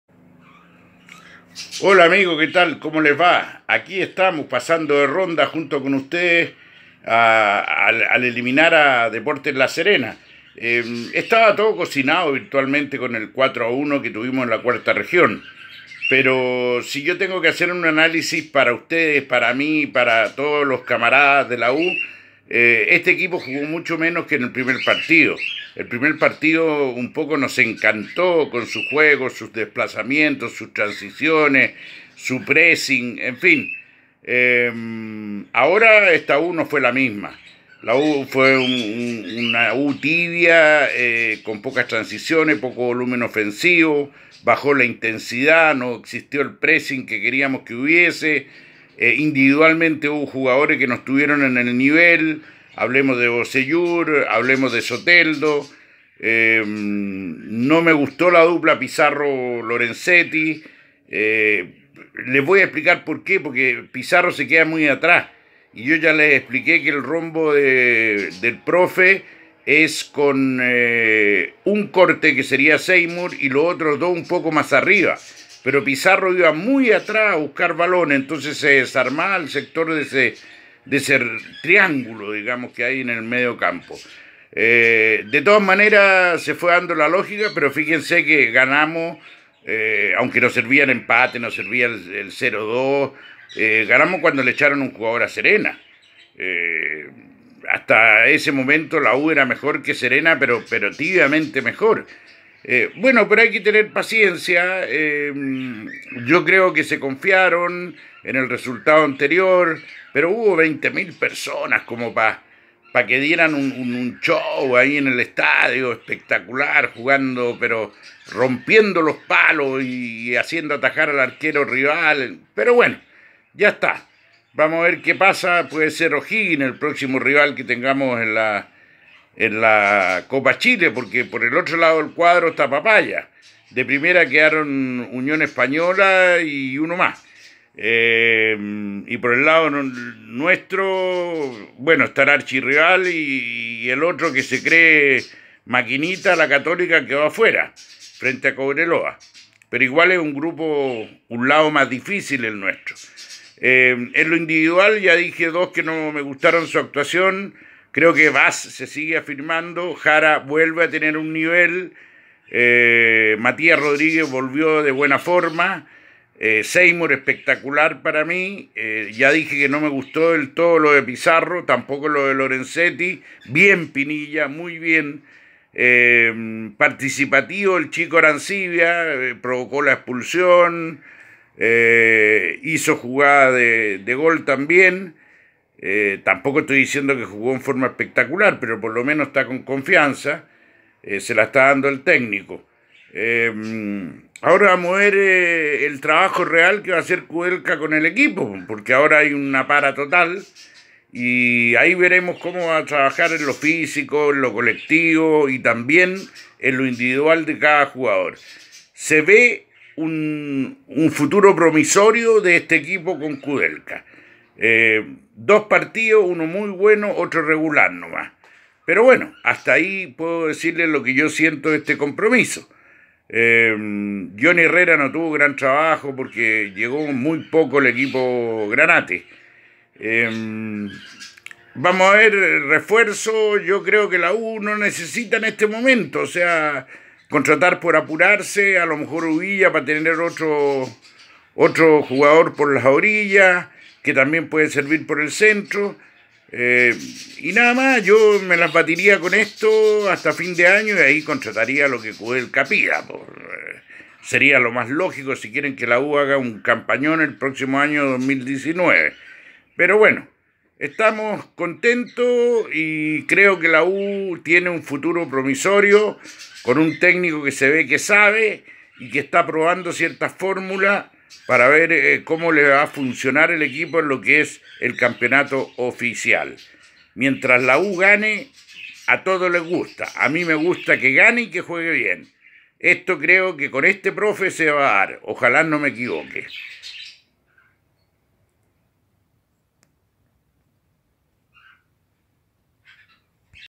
COMENTARIO TITO AWAD: U DE CHILE 2 LA SERENA 0
Nuestro querid Tito nos cuenta sobre la clasificación de la U a la siguiente fase de Copa Chile.